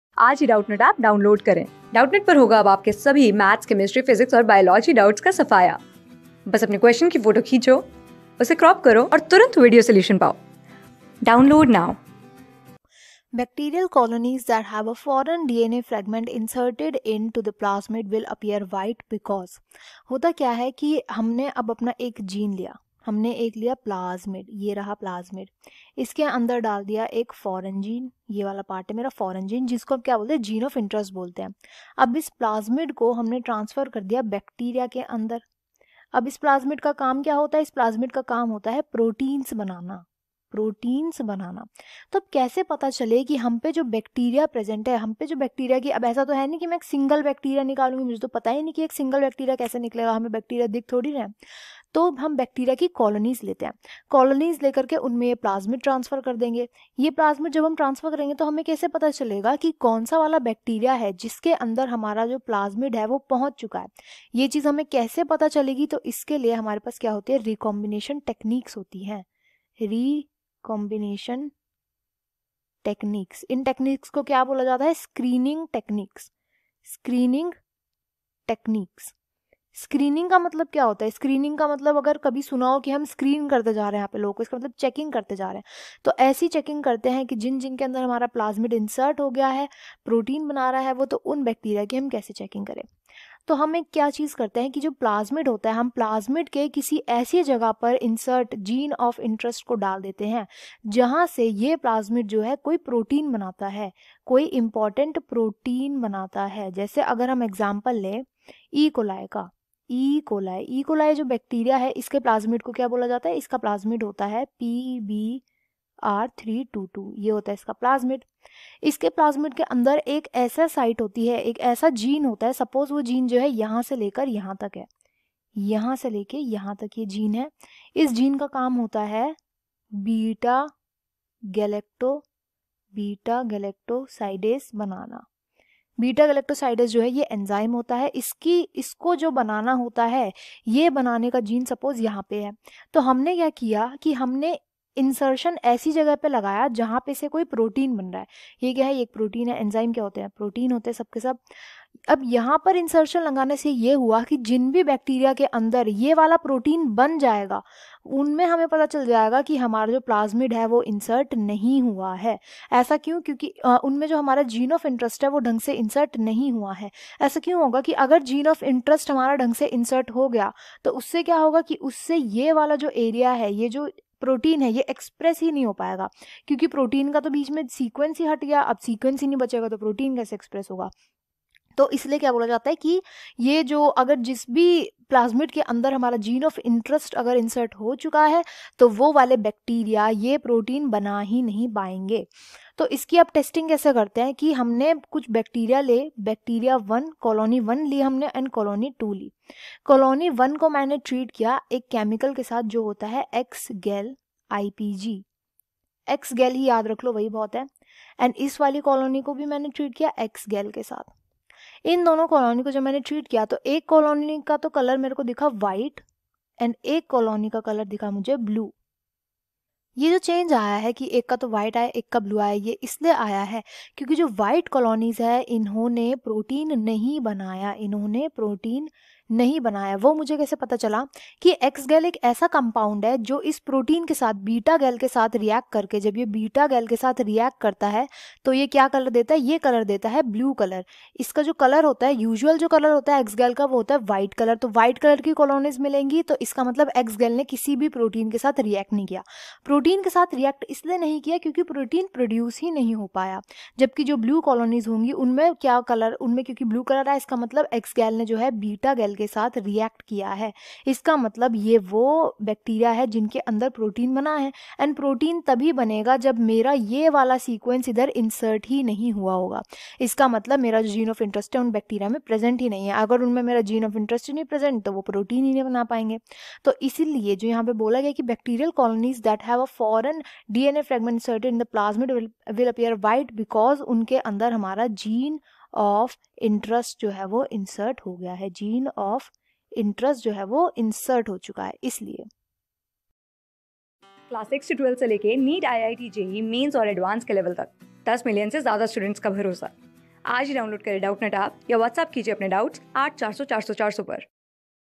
Bacterial colonies that have a foreign DNA fragment inserted in to the plasmid will appear whit...
Bacterial colonies that have a foreign DNA fragment inserted in to the plasmid will appear white because-
Class: 12
Subject: BIOLOGY
Chapter: BIOTECHNOLOGY : PRINCIPLES AND PROCESSES
Board:USER_ASKED

👉 Have Any Query? Ask Us.